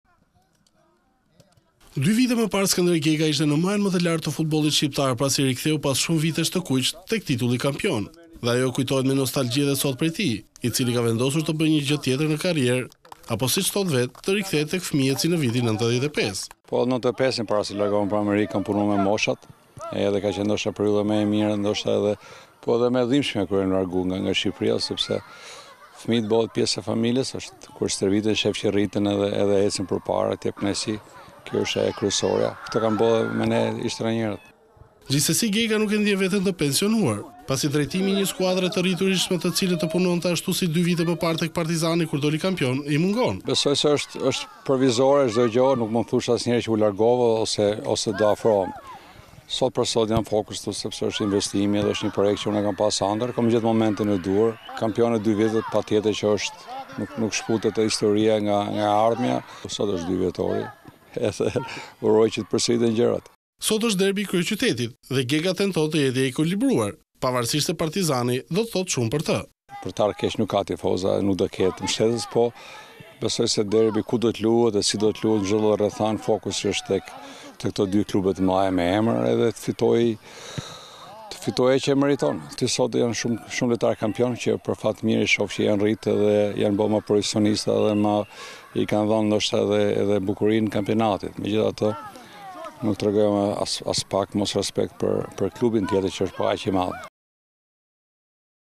Top Channel/ Gega rikthehet te fëmijët/ Ish-trajneri kthehet tek e ardhmja e futbollit
Dy vite më parë Skënder Gega ishte në majën më të lartë të futbollit shqiptar pasi i riktheu pas shumë vitesh të kuqve titullin kampion dhe ajo kujtohet me nostalgji edhe sot prej tij i cili ka vendosur të bëj një tjetër gjë me karrierën e tij, apo sic thotë vetë, të rikthehet tek fëmijët si në vitin 1995.

Skënder Gega: Në vitin 1995 përpara se të largohesha për në Amerikë kam punuar me moshat dhe ka qenë një nga periudhat më të mira të jetës sime dhe më e dhimbshmja kur u largova sepse fëmijët të bëhen pjesë e jetës. Kështu kanë bërë edhe profesorët tanë me ne.

Gjithsesi Gega nuk e ndjen veten të pensionuar, pasi drejtimi i një skuadre të rriturish me të cilën të punojë ashtu si dy vite më parë me Partizanin kur doli kampion, i mungon.

Skënder Gega: Është provizore çdo gjë, nuk mund të them u largova apo do afrohem, këtë gjë e kam pas ëndërr dhe kam gjetur momentin e duhur. Ajo që ndodhi dy vite më parë është pjesë e historisë e kujtoj me nostalgji, nuk shkëputet e shkuara nga e ardhmja. Uroj që të përsëriten gjërat.

Sot luhet derbi i kryeqytetit dhe Gega mbetet i njëanshëm, pasi Partizani do të thotë shumë për të.

Skënder Gega: Të vjen keq që nuk ka tifoza, por derbi është derbi kudo të luhet sido të luhet fokusi mbetet tek këto dy skuadra të mëdha. Të fitojë ai që e meriton. Aty ka shumë lojtar kampion që janë rritur dhe i kanë dhënë bukuri kampionatit, megjithatë nuk duhet të harrojmë të respektojmë skuadrën tjetër se është po kaq e madhe.